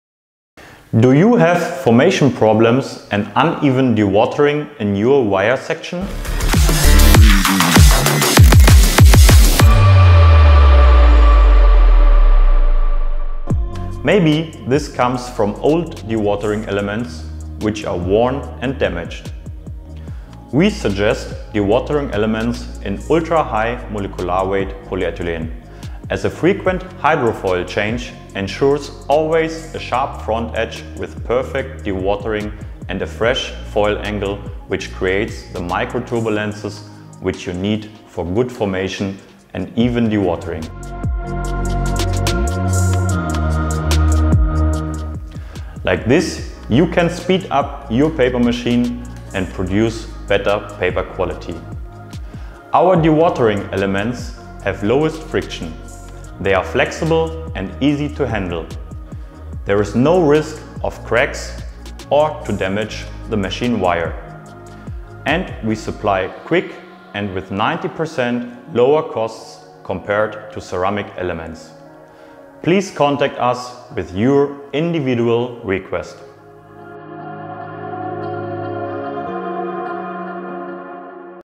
Dewatering elements made of flexible & wear-resistant UHMW-PE
Meet the solution to your formation problems and uneven dewatering in the wire section of your machine. Dewatering elements made from UHMW-PE have lowest friction, are flexible and easy to handle. They effectively reduce the risk of cracks or damage to the machine wire. Our high-quality dewatering elements are the perfect alternative solution to ceramic elements.